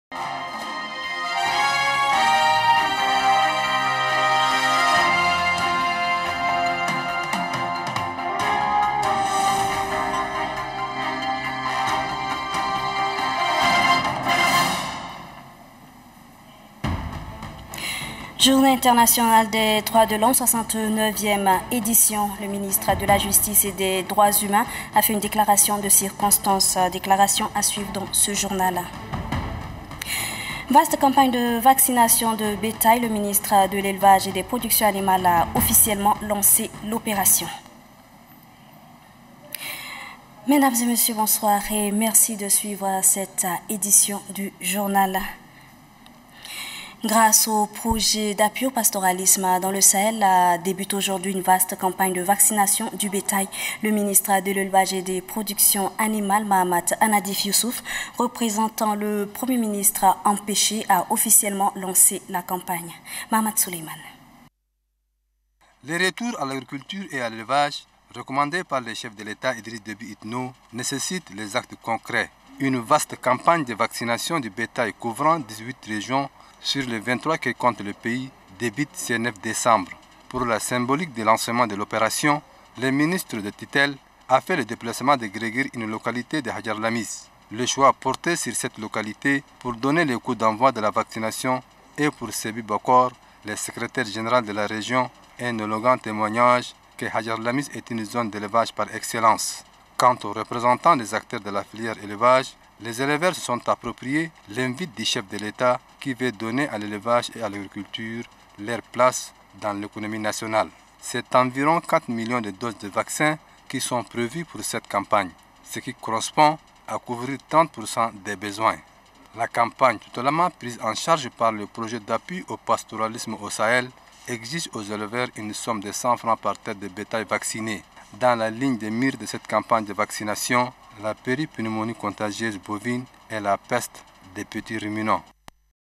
INFOS TELE TCHAD DU 09 DECEMBRE 2017 - Elevage
Titres
1) Titres du journal.
2) Vaccination contre la péripreumonie bovine et peste.